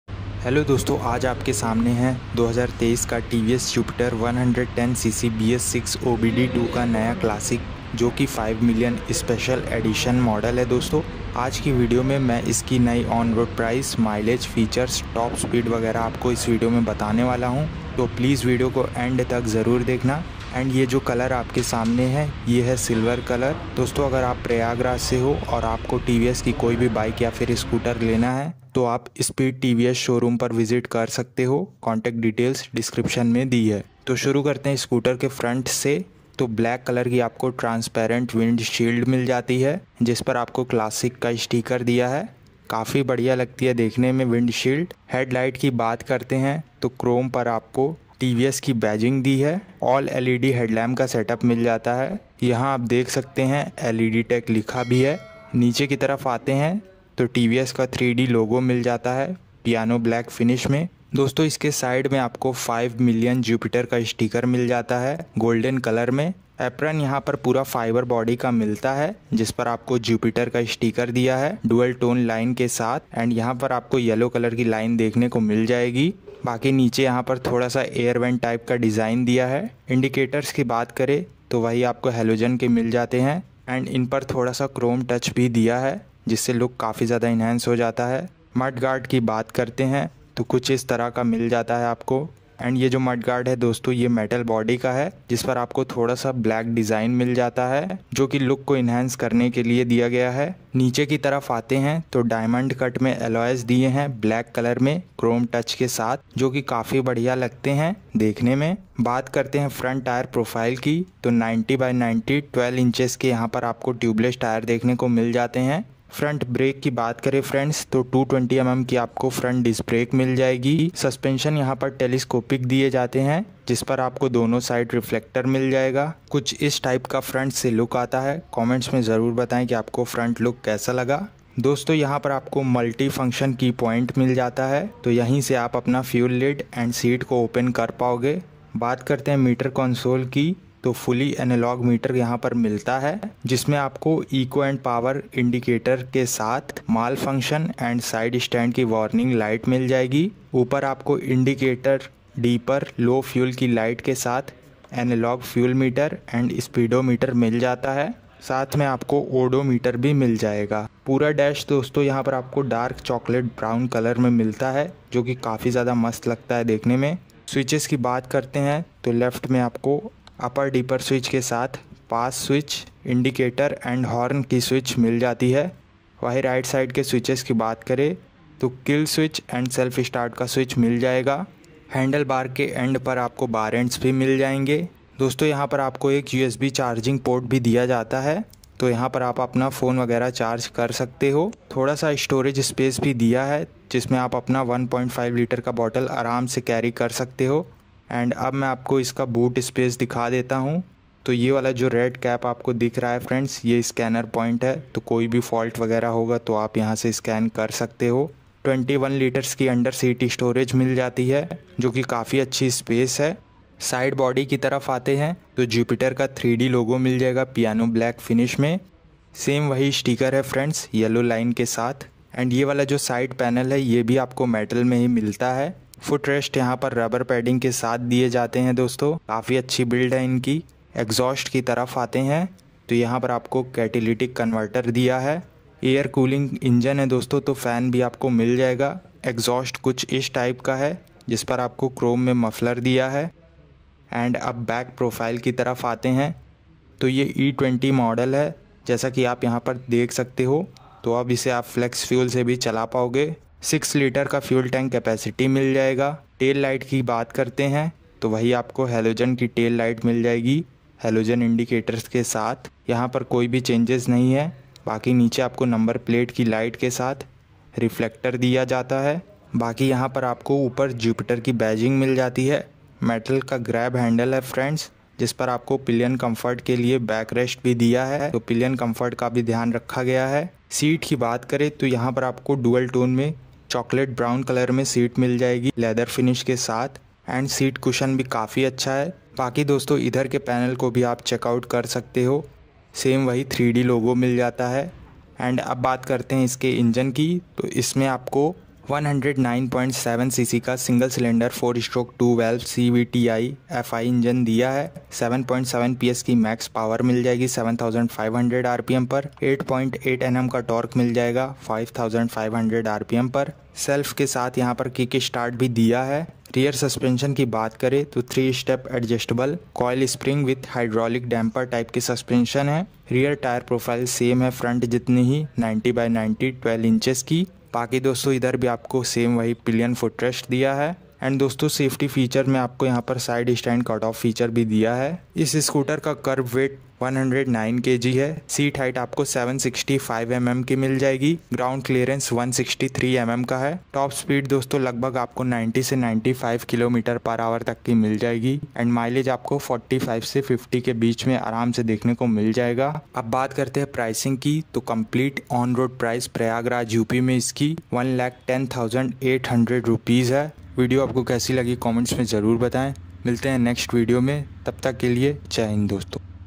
2023 New Jupiter classic 110 Bs7 E-20 | New Update | Price Mileage Full Detail Review
Showroom Details- Speed TVS,
Mahatma Gandhi Marg, near Harsh Hotel, Civil Lines, Prayagraj, Uttar Pradesh 211001